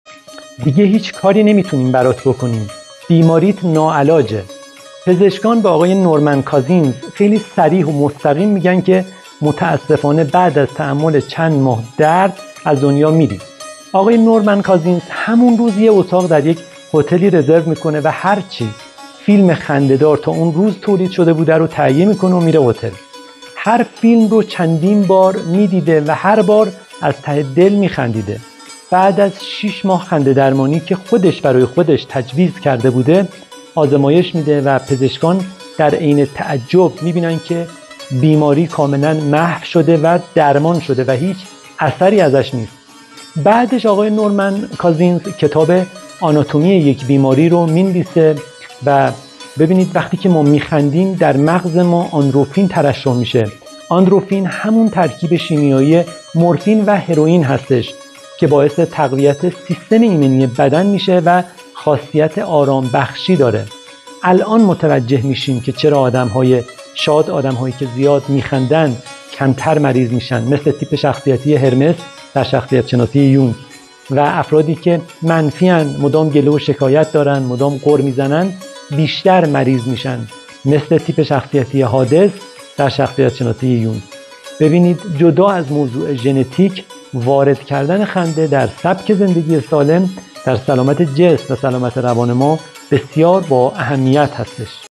دکترا جوابش کردن
❌ تجربه واقعی ❌
نجات زندگی آقای نورمن کازینز توسط خودش
آقای نورمن کازینز چون به بیماری ناعلاج مبتلا شده بوده، به مدت شش ماه خودش برای خودش خنده درمانی تجویز می کنه
و نهایتا بیماریش بهبود پیدا می‌کنه.
حتما ویدیو رو ببینید.
چرا آدم‌های شاد کمتر مریض میشن؟
مثل تیپ شخصیتی هرمس
چرا آدم‌های منفی و غمگین بیشتر مریض میشن؟
مثل تیپ شخصیتی هادس
هرمس و هادس دوتا از پانزده تیپ شخصیتی یونگ هستند.